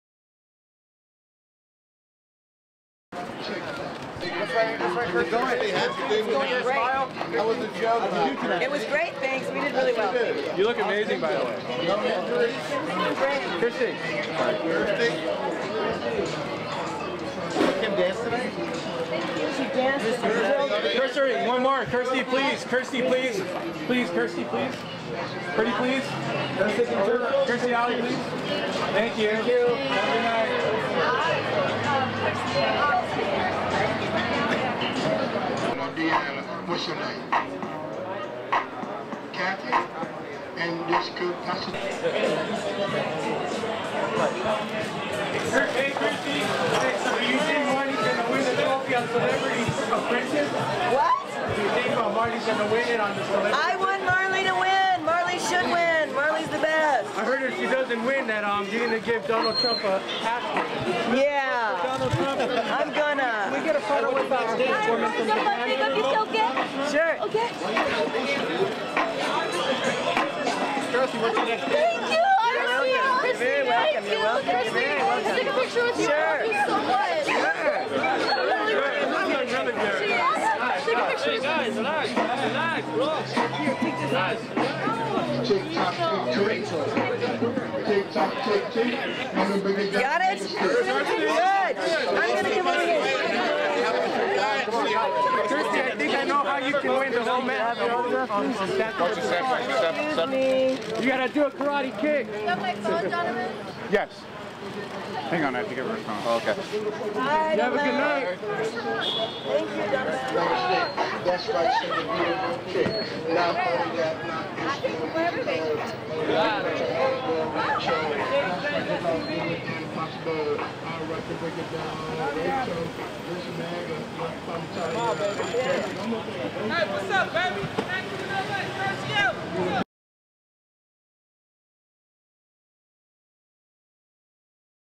Kirstie Alley Sushis Out at Katsuya After DWTS W051611
E.L.Woody reports 051611
Got Kirstie coming and going at Katsuya in Hollywood. I think her secret of staying thin, and losing weight, must be eating lots of sushi. She eats here after every Monday night's DWTS competition.